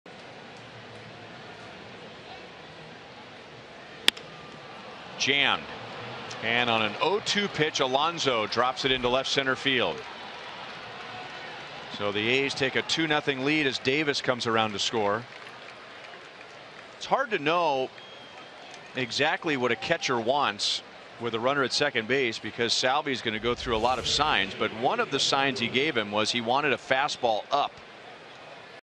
OAK@KC: Alonso drops one into the gap for an RBI
With two runners on, Yonder Alonso drops a base hit into the left-center-field gap, plating Khris Davis and advancing Stephen Vogt to third